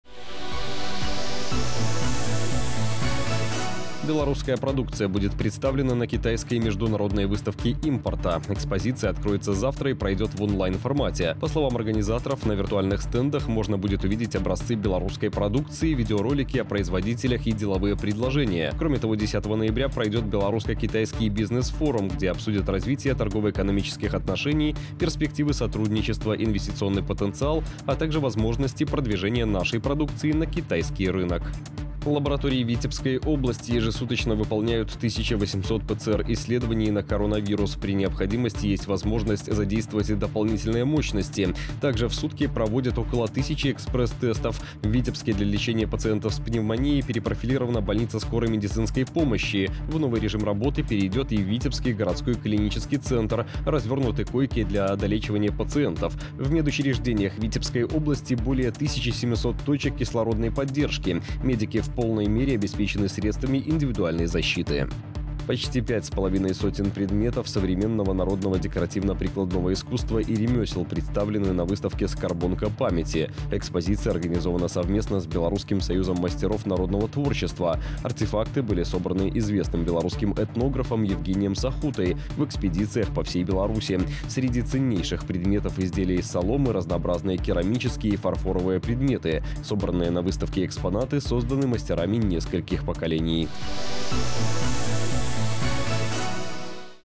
Новости Беларуси 4 ноября 2020 года
Белорусская продукция будет представлена на Китайской международной выставке импорта. Экспозиция откроется завтра и пройдёт в онлайн-формате. По словам организаторов, на виртуальных стендах можно будет увидеть образцы белорусской продукции, видеоролики о производителях и деловые предложения. Кроме того, 10 ноября пройдёт белорусско-китайский бизнес-форум, где обсудят развитие торгово-экономических отношений, перспективы сотрудничества, инвестиционный потенциал, а также возможности продвижения нашей продукции на китайский рынок.

Лаборатории Витебской области ежесуточно выполняют 1800 ПЦР-исследований на коронавирус. При необходимости, есть возможность задействовать дополнительные мощности. Также в сутки проводят около тысячи экспресс-тестов. В Витебске для лечения пациентов с пневмонией перепрофилирована больница скорой медицинской помощи, в новый режим работы перейдёт и витебский городской клинический центр, развёрнуты койки для долечивания пациентов. В медучреждениях Витебской области более 1700 точек кислородной поддержки. Медики в полной мере обеспечены средствами индивидуальной защиты.